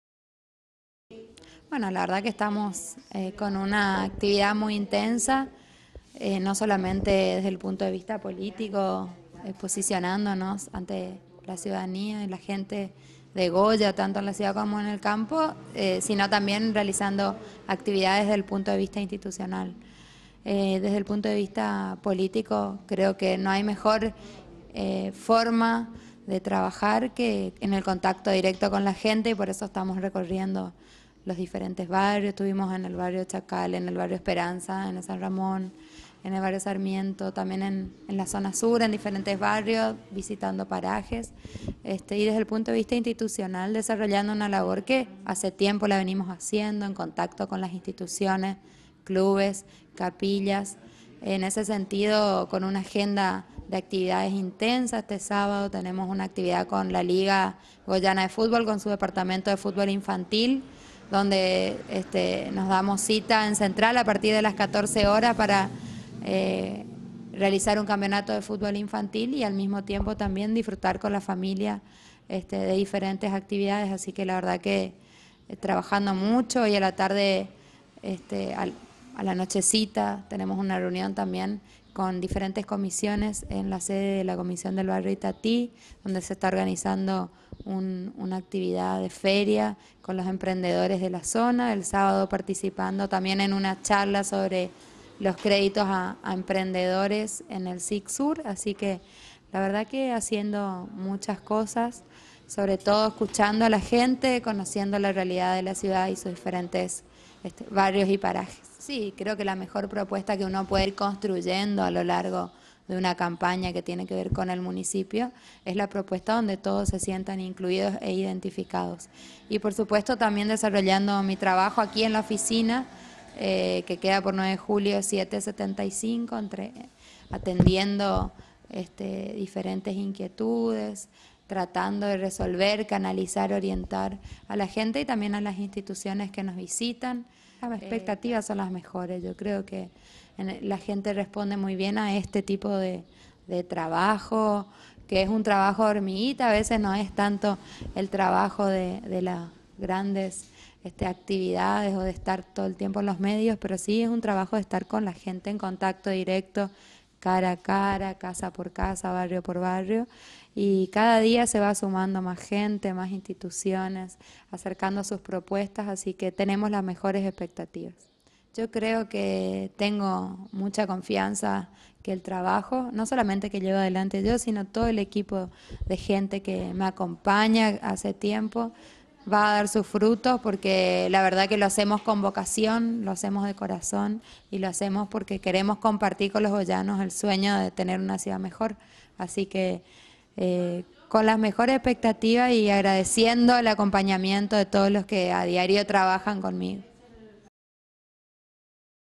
Como se esta trabajando
Cecilia Gortari, candidata a intendente de Goya por la U.C.R.